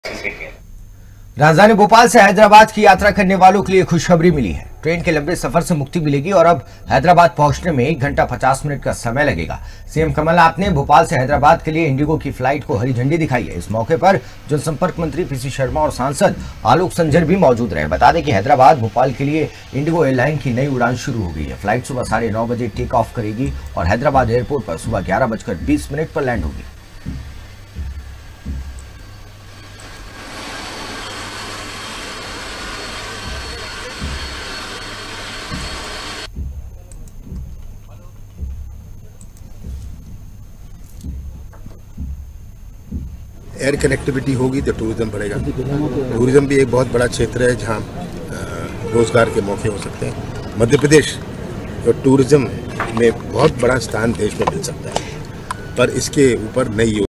भोपाल से हैदराबाद के लिए इंडिगो की फ्लाइट सेवाएं शुरु
मध्यप्रदेश की राजधानी भोपाल से देश के अन्य शहरों की एयर कनेक्टिविटी को बढ़ाते हुए शनिवार को मुख्यमंत्री कमलनाथ ने राजाभोज एयरपोर्ट से इंडिगो एयरलाइंस के संचालन का उद्घाटन किया। इस मौके पर सांसद अलोक संजर, विधायक रामेश्वर शर्मा शामिल हुए।